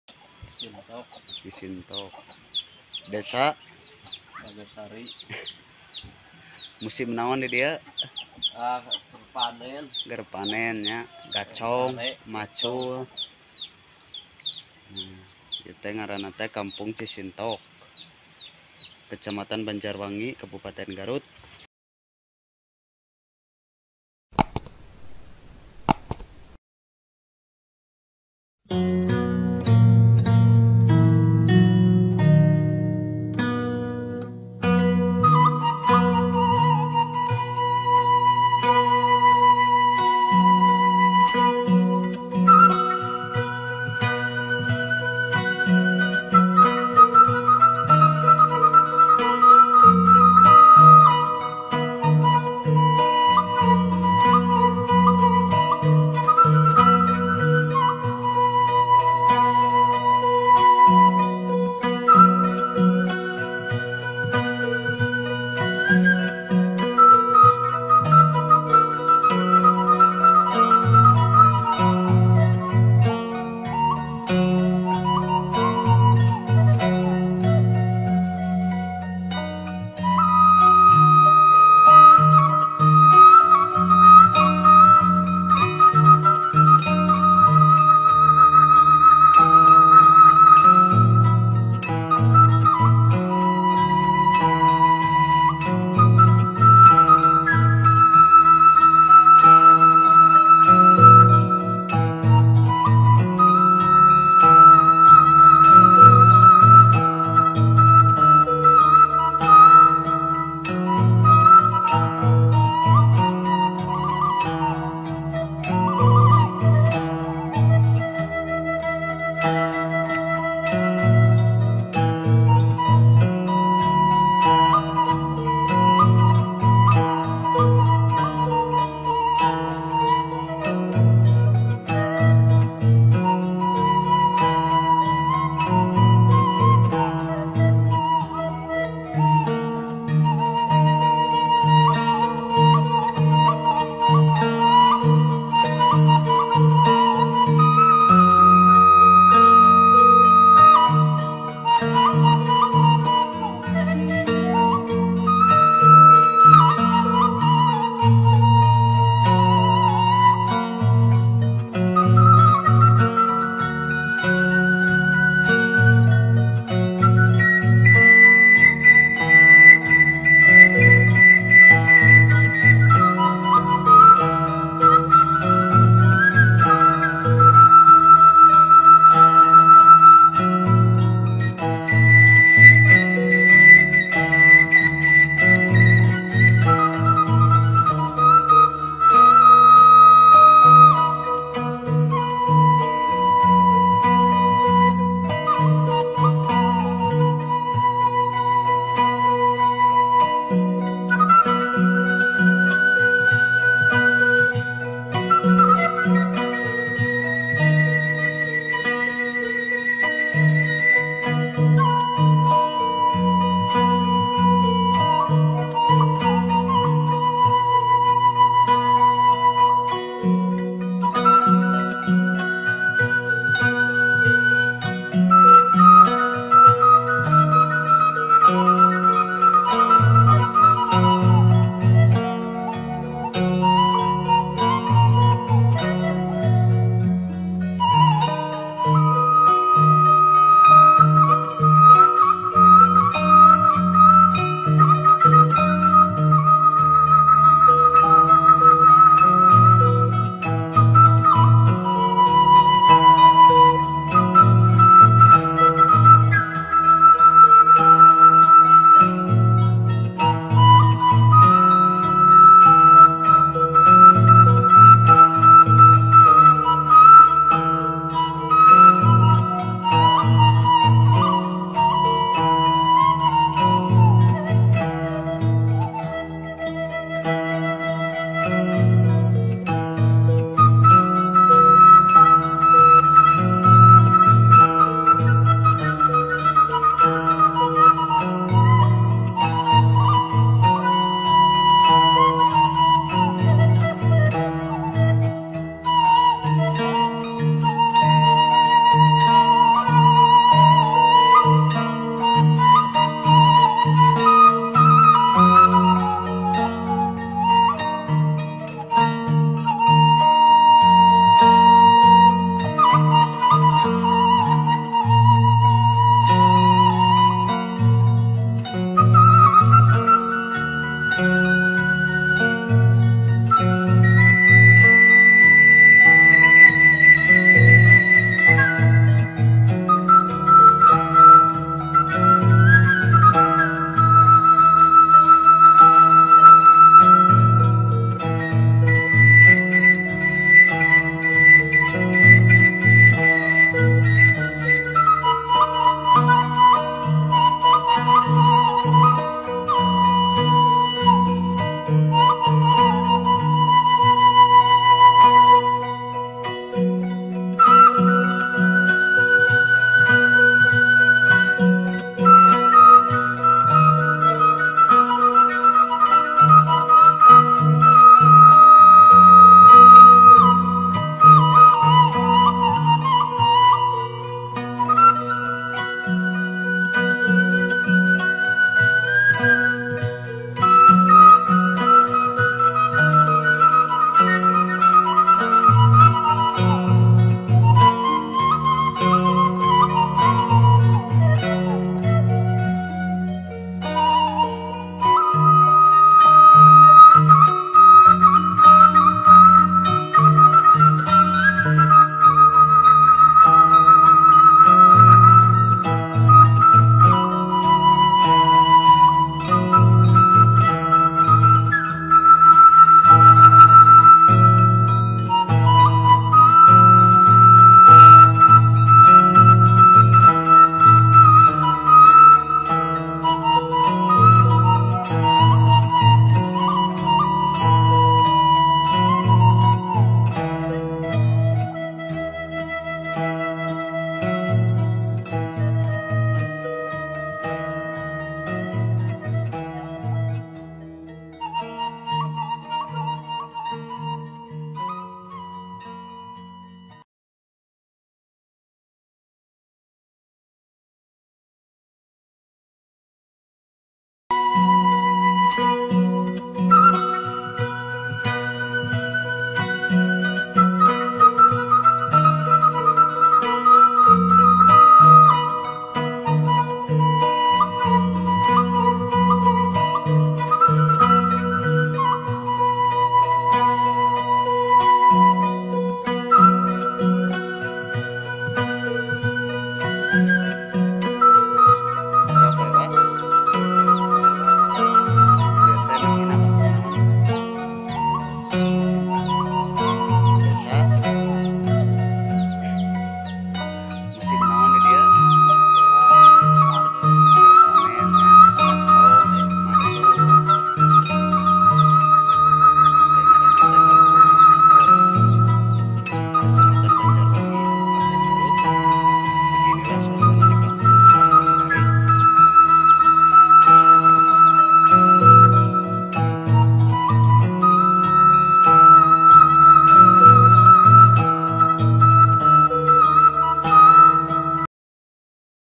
beautiful village atmosphere friendly villagers, Sundanese rural atmosphere Banjarwangi
this time I explored the atmosphere of the Sundanese countryside in the cisintok village of Talagasari village where the atmosphere was cool and beautiful plus the activities of the residents in the morning plus the people were friendly,
in the district of Banjarwangi, Garut Regency, West Java Province, Indonesia.
The distance from Banjarwangi to the capital city of Garut Regency is around 50 km. Banjarwangi Most of its areas include forestry, mixed plantations, rice fields, fields, gardens and so on.
and Banjarwangi are bordered by the regencies of Tasikmalaya, Bayongbong, Cigedug, Cilawu, Cikajang, Cihurip and Singajaya.